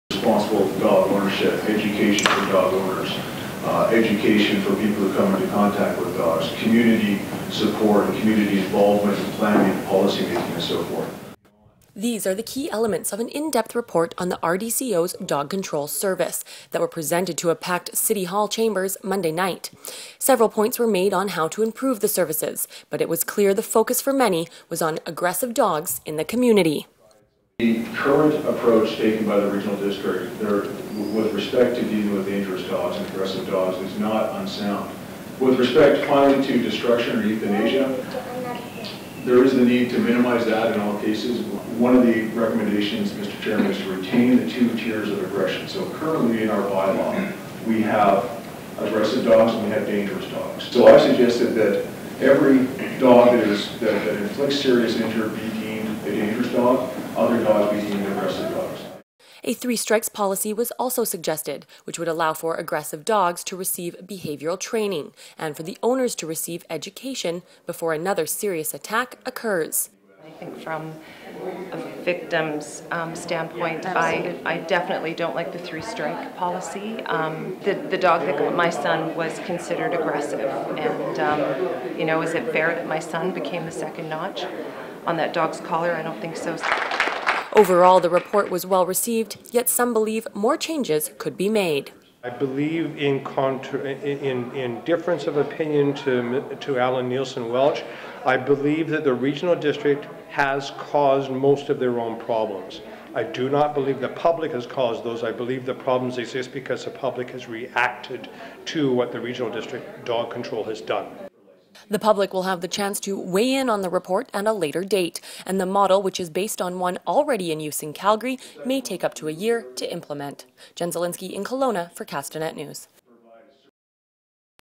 Dog control model supported
RDCO and members of the public support a new dog control model. (Video: Jen Zielinski)